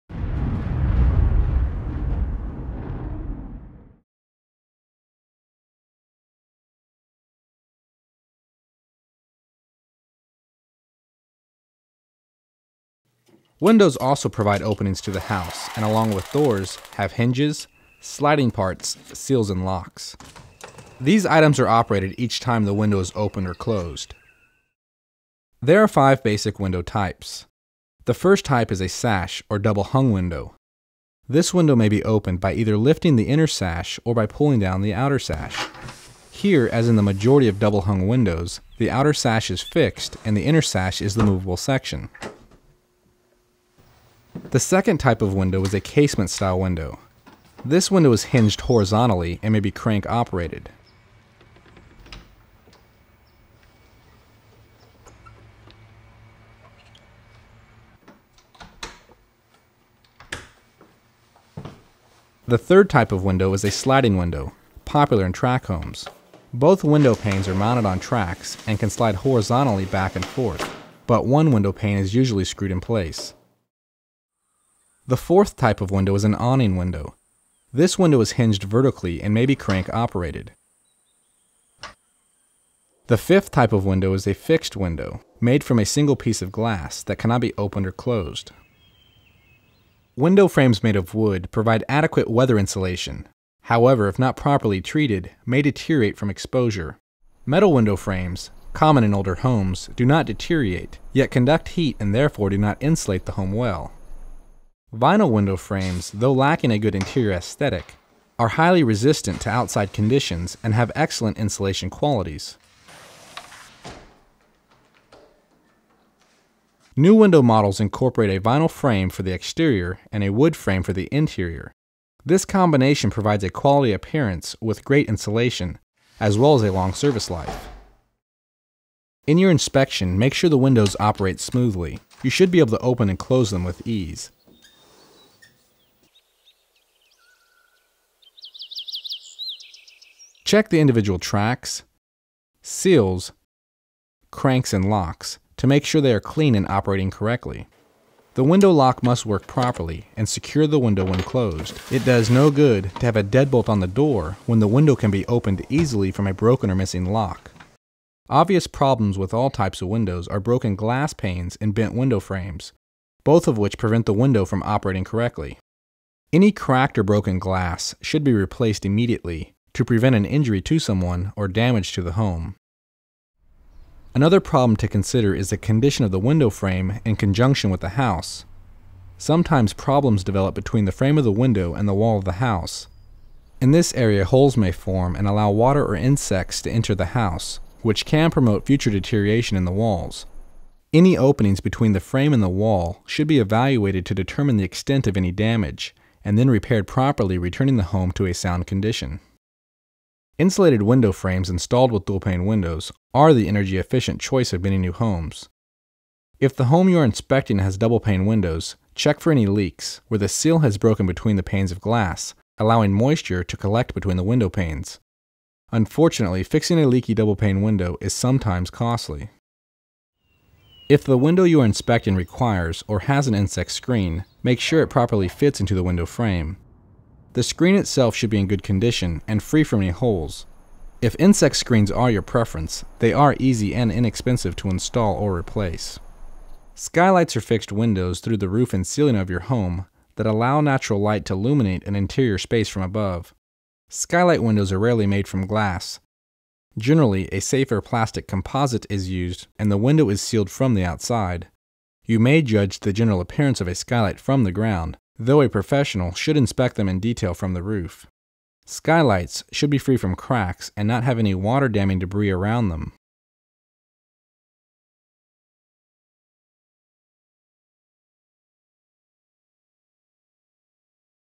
Home Inspection - Windows
Windows also provide openings to the house and along with doors have hinges, sliding parts, seals, and locks.  These items are operated each time the window is opened or closed.

Window frames made of wood provide adequate weather insulation, however, if not properly treated, may deteriorate from exposure.  Metal window frames, common in older homes do not deteriorate, yet conduct heat and therefore do not insulate the home well.  Vinyl window frames, though lacking a good interior aesthetic, are highly resistant to outside conditions and have excellent insulation qualities.  New window models incorporate a vinyl frame for the exterior and wood frame for the interior.  This combination provides a quality appearance with great insulation, as well as a long service life.

Types Windows

There are five basic window types.  The first window type is a sash, or double hung window.  This window may be opened either by lifting the inner sash or by pulling down the outer sash.  Here, as in the majority of double hung windows, the outer sash is fixed and the inner sash is the moveable section.  The second type of window is a casement style window.  This window is hinged horizontally and may be crank operated.  The third type of window is a sliding window, popular in track homes.  Both windowpanes are mounted on tracks a can slide horizontally back and forth, but one windowpane is usually screwed in place.  The forth type of window is the awning window.  This window is hinged vertically and may be crank operated.  The fifth type of window is a fixed window, made from a single piece of glass that cannot be opened or closed.

In your inspection, make sure the windows operate smoothly.  You should be able to open and close them with ease.  Check the individual tracks, seals, cranks, and locks, to make sure they are clean and operating correctly.  The window lock must work properly and secure the window when closed.  It does no good to have a dead bolt on the door when a window can be opened easily from a broken or missing lock.  Obvious problems with all types of windows are broken glass panes and bent window frames, both of which prevent the window from operating correctly.

Any cracked or broken glass should be replaced immediately to prevent an injury to someone or damage to the home.  Another problem to consider is the condition of the window frame in conjunction with the house.  Sometimes problems develop between the frame of the window and the wall of the house.  In this area, holes may form and allow water or insects to enter the house, which can promote further deterioration in the walls.  Any openings between the frame and the wall should be evaluated to determine the extent of any damage and then repaired properly, returning the home to a sound condition.

Insulated window frames installed with dual pane windows are the energy efficient choice of many new homes.  If the home you are inspecting has double pane windows check for any leaks where the seal has broken between the panes of glass, allowing moisture to collect between the windowpanes.  Unfortunately, fixing a leaky double pane window is sometimes costly.

If the window you are inspecting requires or has an insect screen, make sure it properly fits into the window frame.  The screen itself should be in good condition and free from any holes.  If insect screens are your preference they are easy and inexpensive to install or replace.

Skylights are fixed windows, through the roof and ceiling of your home, that allow natural light to illuminate an interior space from above.  Skylight windows are rarely made from glass; generally a safer plastic composite is used and the window is sealed from the outside.  You may judge the general appearance of a skylight from the ground, though a professional should inspect them in detail from the roof.  Skylights should be free of cracks and not have any water damming debris around them.